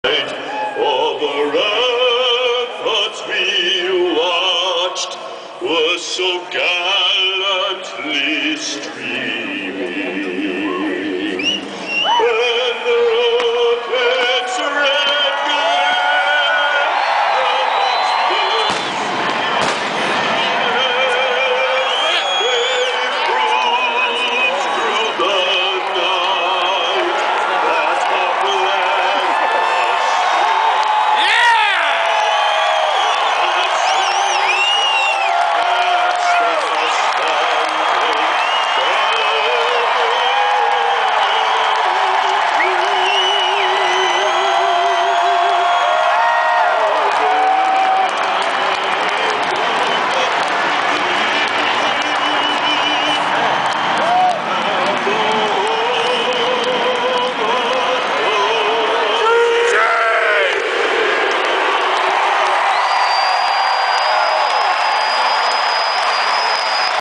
Stealth bomber flyover at Arrowhead stadium
Stealth bomber fly over at Arrowhead stadium Broncos at Chiefs November 2007